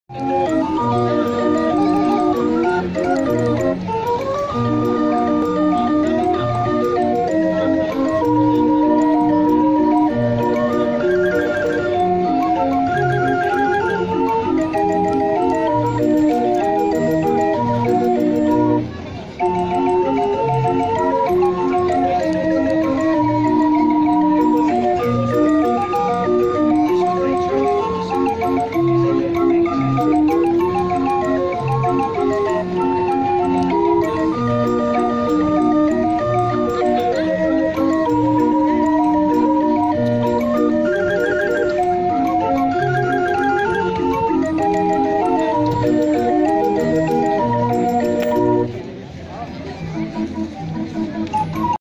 Busking in Hereford
Short clip of busking with a hand turned street organ